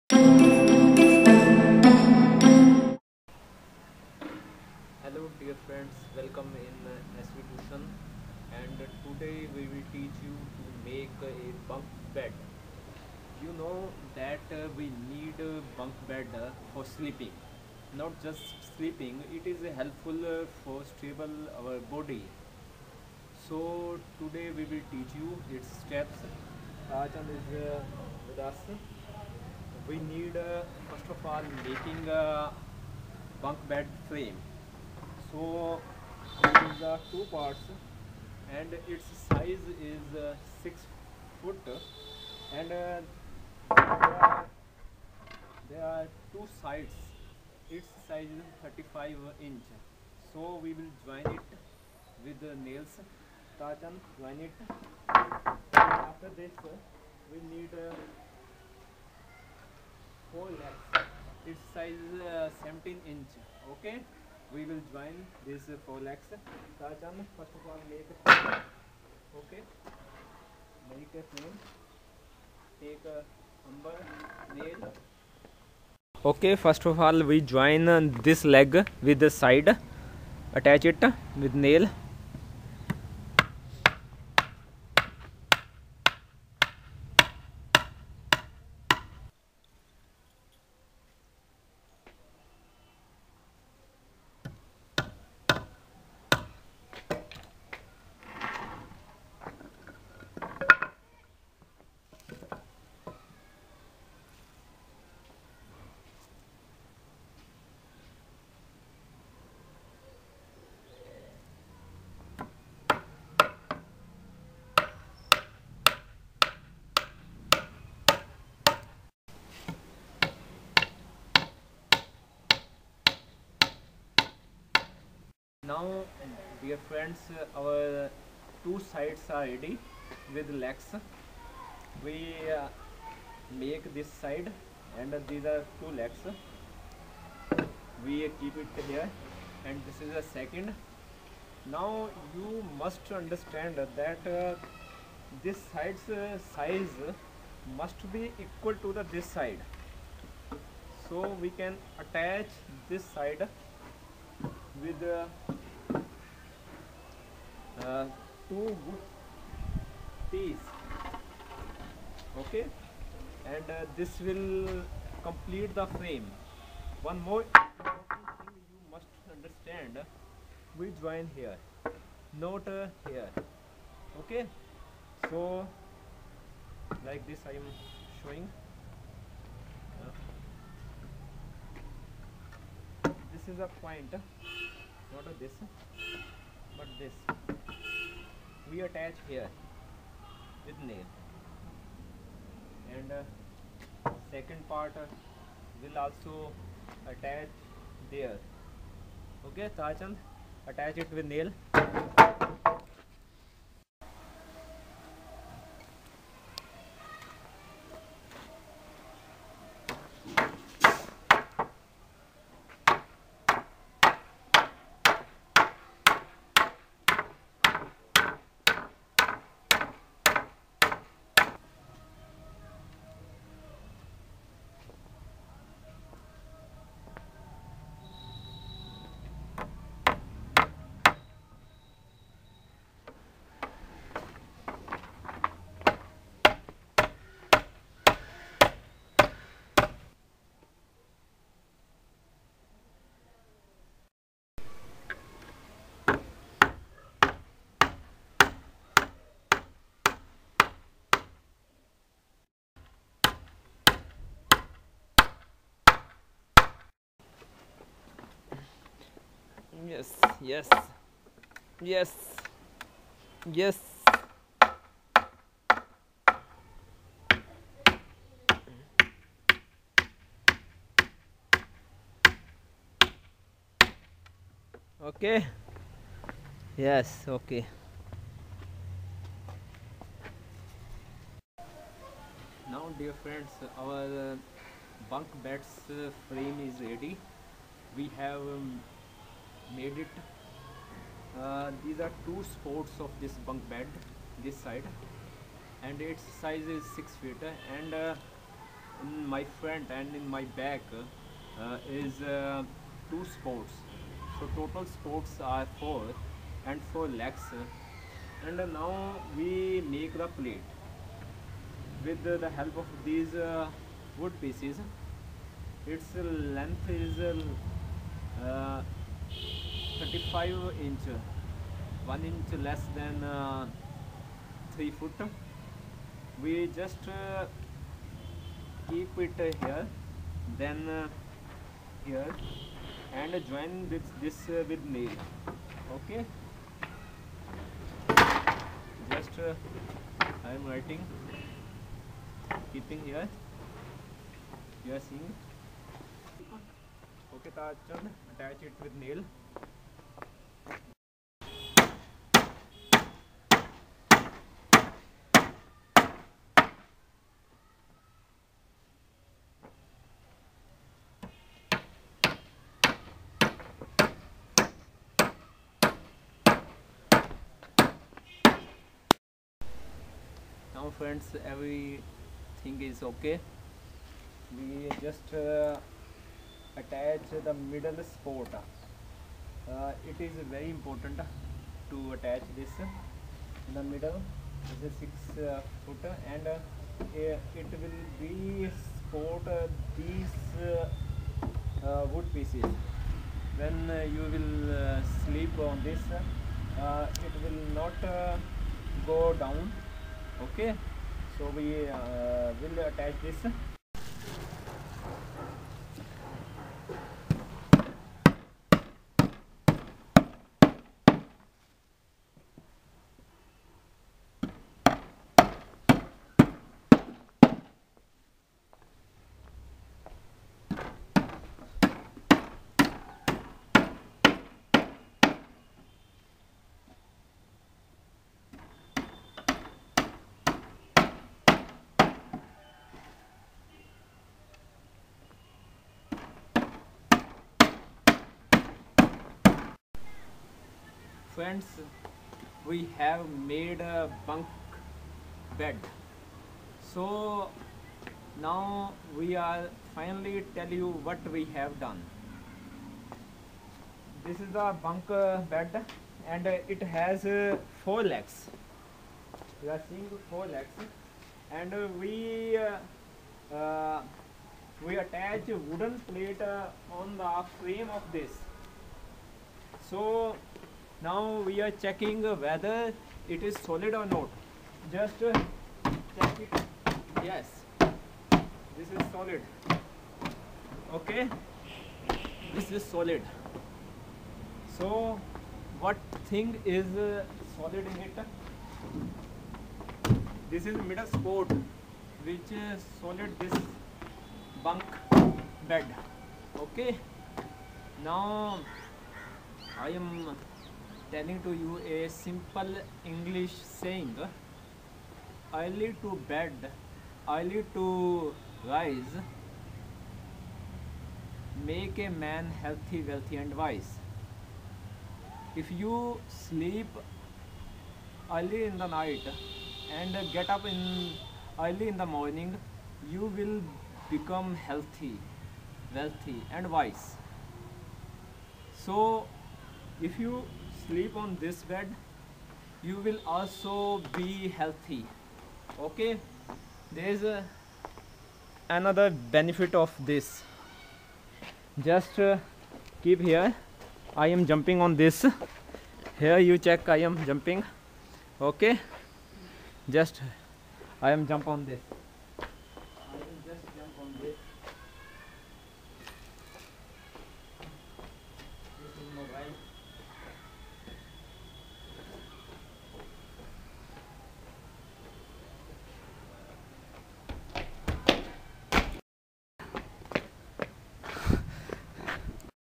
How to Build a Bunk Bed
There are two benefits of building a bunk bed. One, it is helpful to stable human body structure. Just a week, if you will sleep on a bunk bed, you can get relief from different body pains. So, today, we will teach you to build a bunk bed in home. For this, you need three 6 foot lengths' supports. Two 3 foot width support. Both should be of wood. Now, join these supports and make frame of a bunk bed.On this frame attach wooden pieces. Now, you bunk bed is ready.